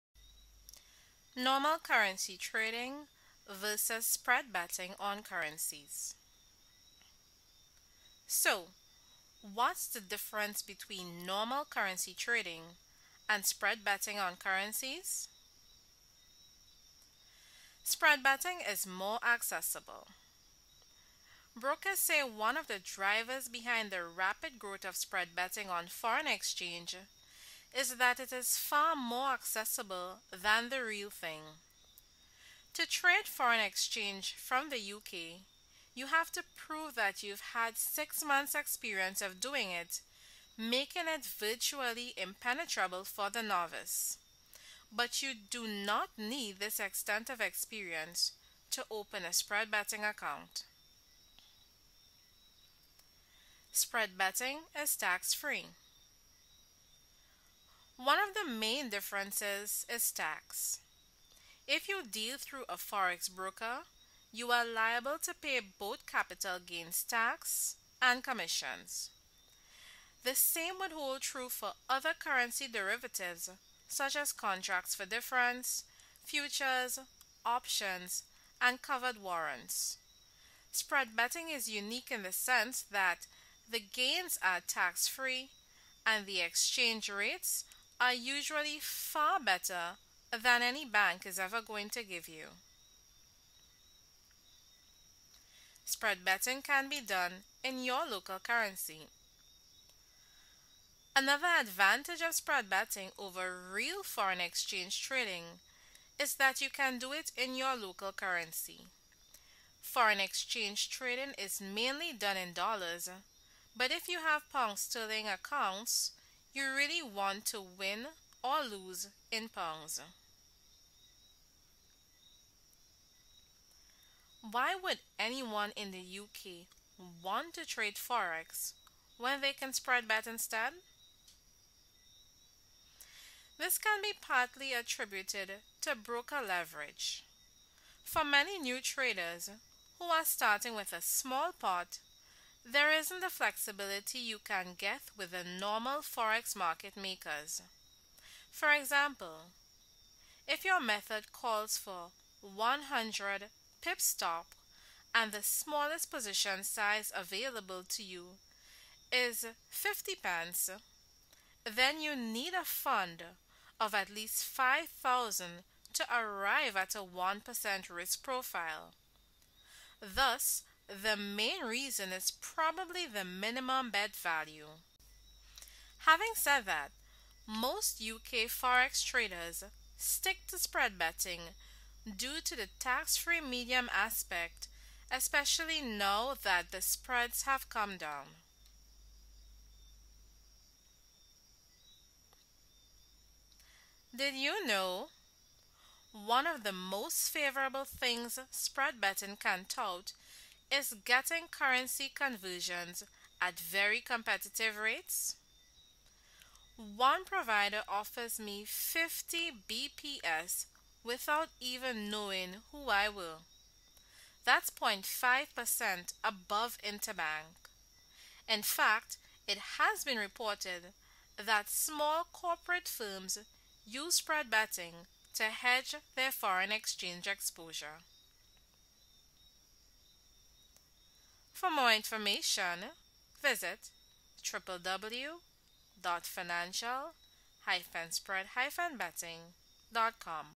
Comparing Spread Betting to Forex Trading
Comparing Spread Betting to Forex Trading On the subject of forex trading, spread betting firms' spreads are very similar to retail forex brokers. That's right. The costs are the same. The difference is whether you get taxed on it which can really make a substantial difference. Makes the decision a no-brainer for anyone other than a consistent loser not troubled by annual profits.

With spread-betting, there are no lots as such. You just decide how much you want to "bet". So, for instance, if you look at EUR/USD the spread will be, say, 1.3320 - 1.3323. Let's say you think EUR/USD will fall, and you want to go short.